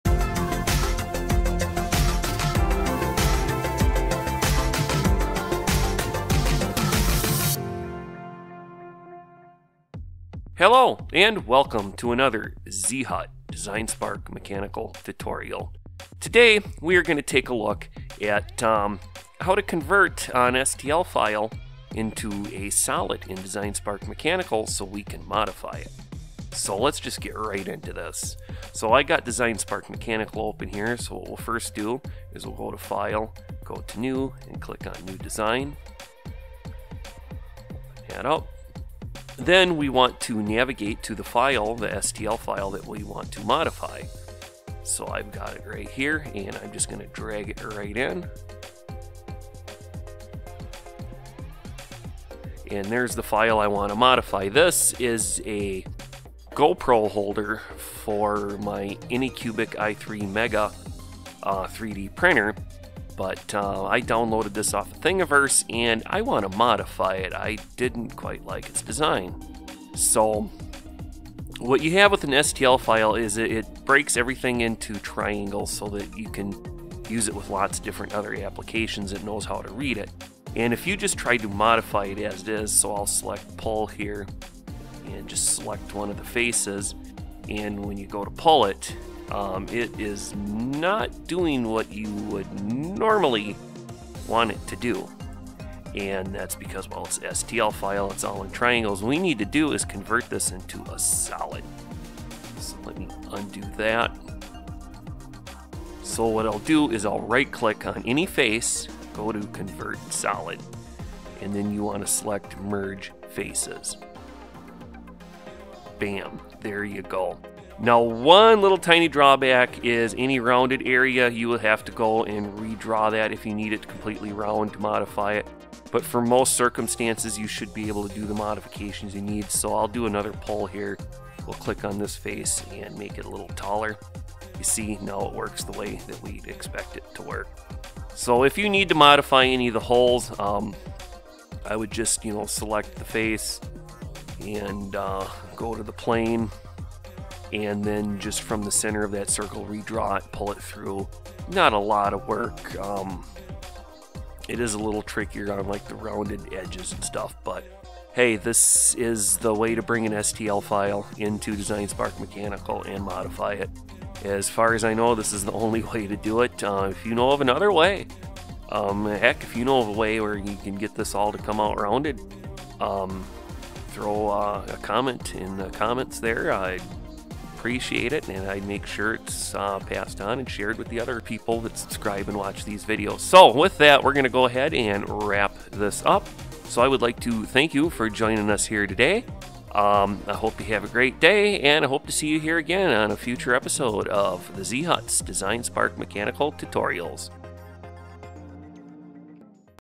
How to convert a .STL to a solid
Today we will take a look at how you can easily convert an STL file into a solid body in designspark mechanical.